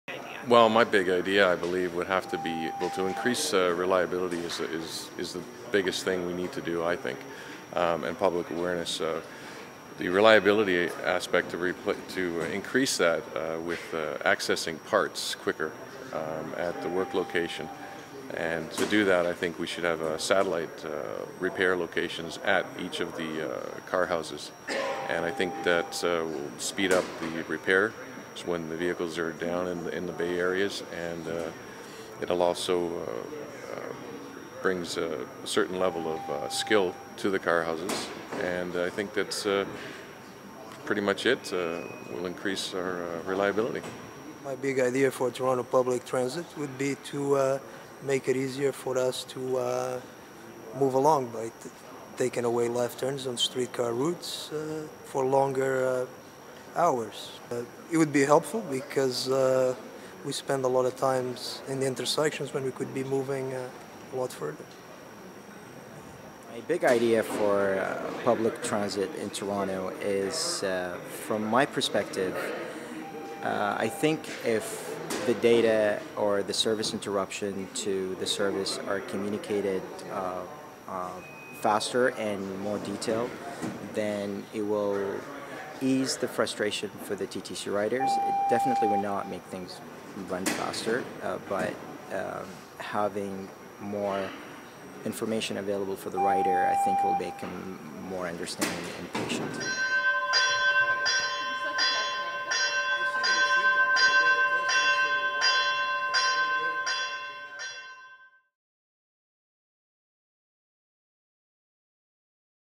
Hidden Experts - Transit
Hidden Experts - Transit officials talk to the Star about their ideas